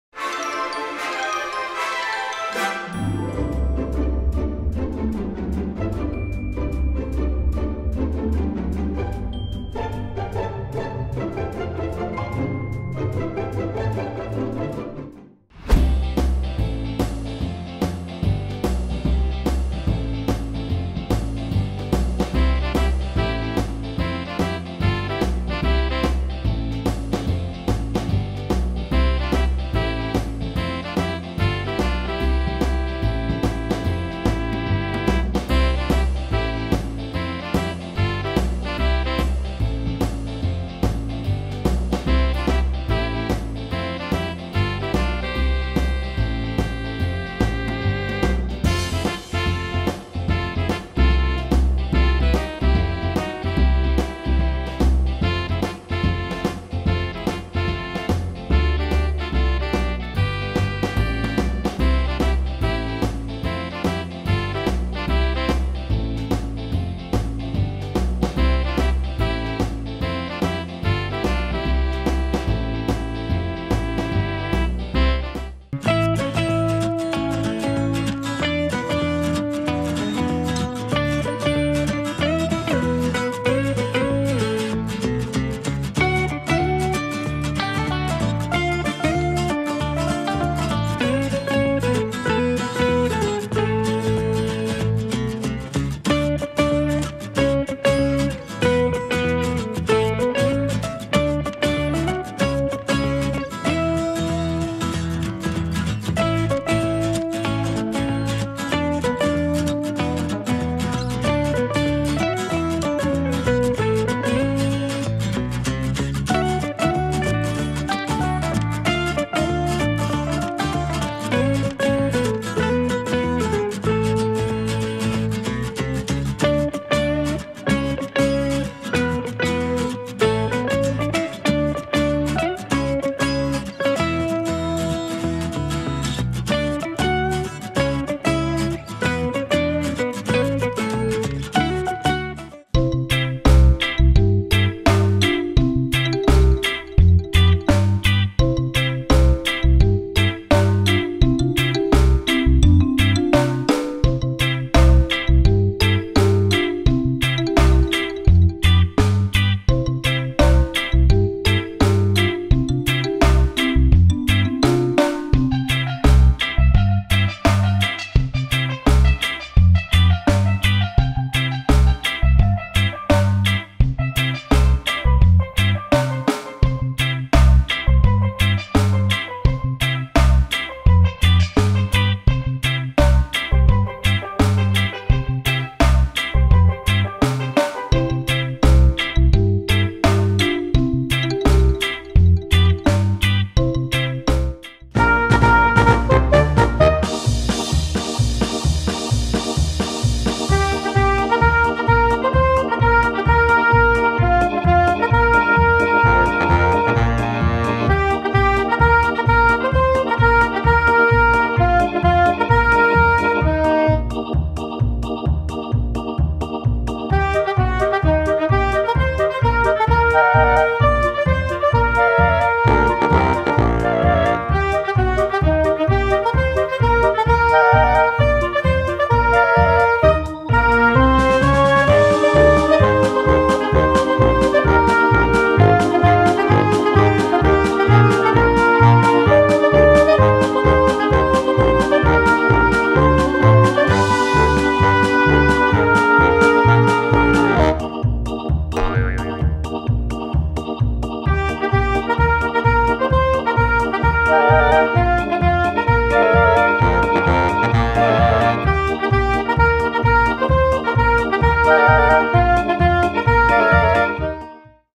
Tower of Pisa hamster maze ⭐ Homura Ham
In this hamster video, we created a Giant 6-level maze - Tower of Pisa! This is a new Obstacle Course for our tiny syrian Hamster. One of the levels is Pool Maze.
Our hamster Joe escapes from the Tower.